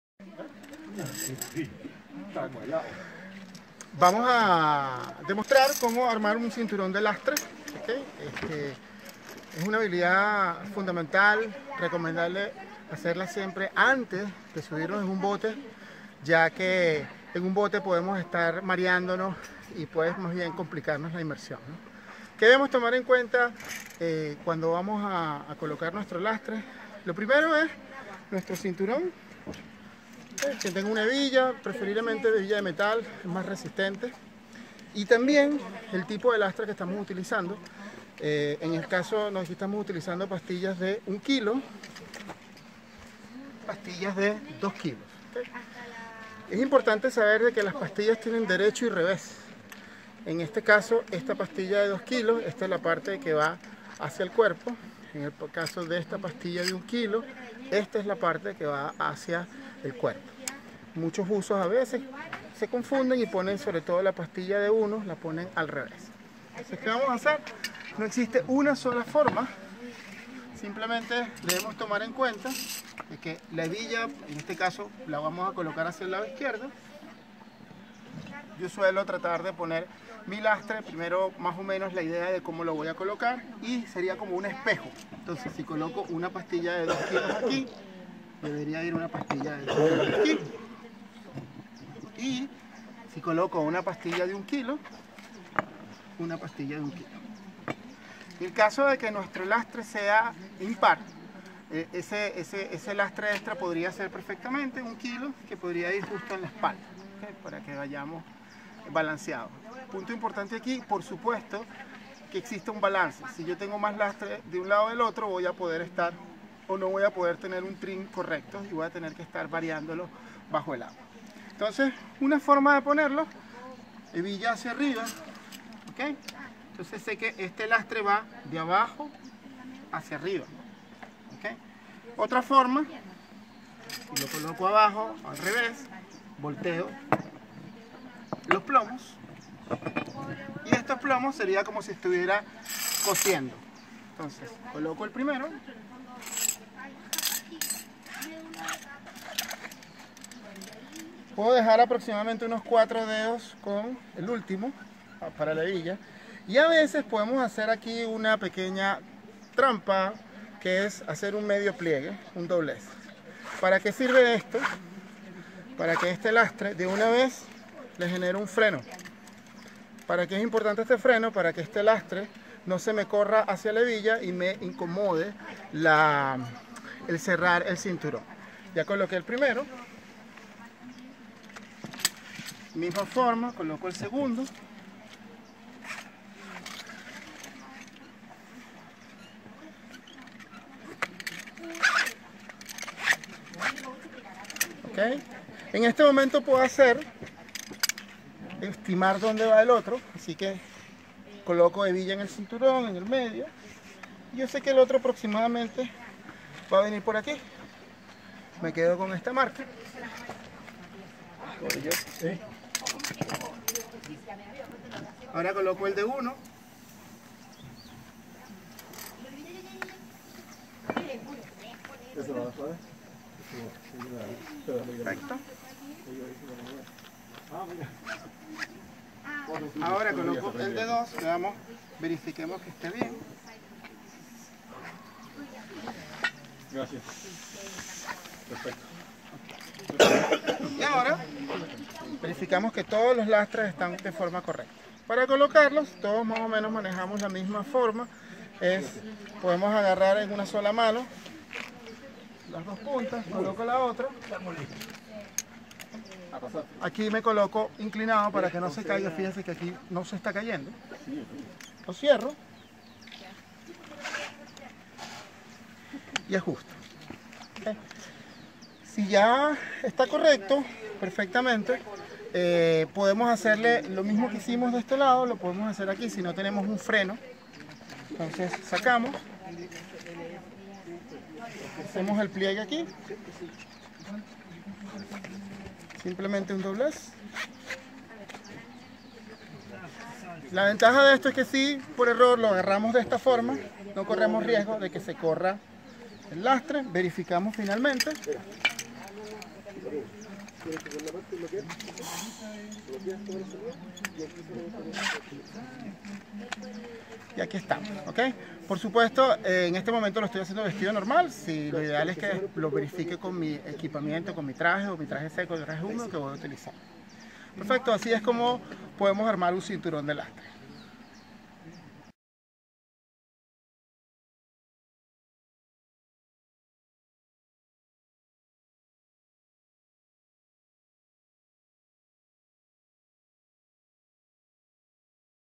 Armando cinturón de lastre
Comparto con ustedes algunos recordatorios de cómo armar tu cinturón de lastre.